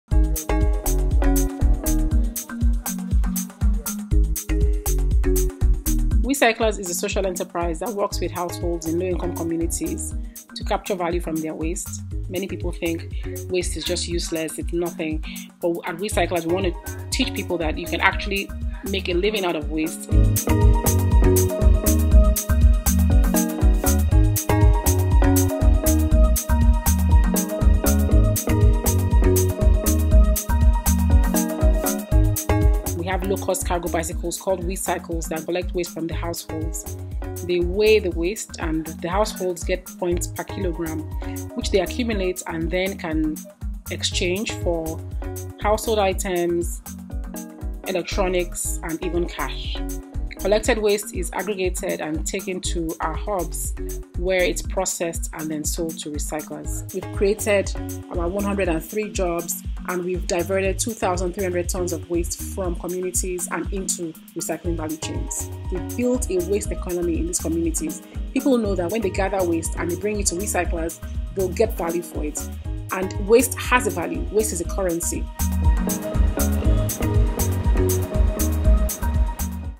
Wecyclers Is Fighting Nigeria's Waste Crisis | HuffPost
Wecyclers gives households a chance to capture value from their waste, while providing a reliable supply of materials to the local recycling industry. It's turning trash into treasure in Nigeria.

HuffPost has launched the Reclaim campaign to highlight the problem of global food waste, and more importantly, work towards reducing it.